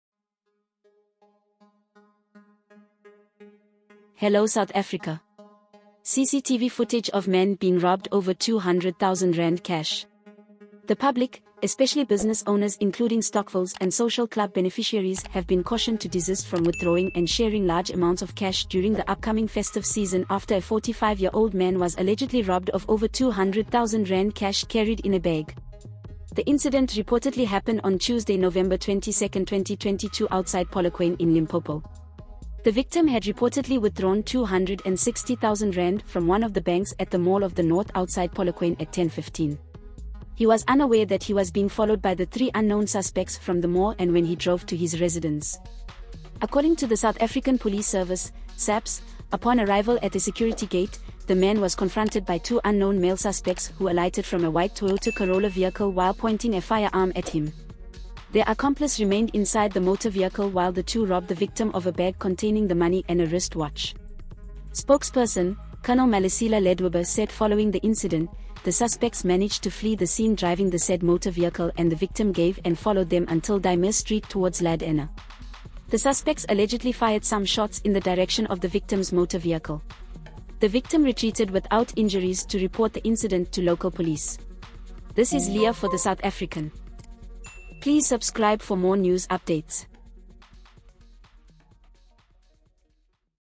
CCTV footage of man being robbed over R200,000 cash | NEWS IN A MINUTE
The public, especially business owners including stokvels and social club beneficiaries have been cautioned to desist from withdrawing and sharing large amounts of cash during the upcoming festive season after a 45-year-old man was allegedly robbed of over 200,000 Rand cash carried in a bag.

Video: Twitter/SAPoliceService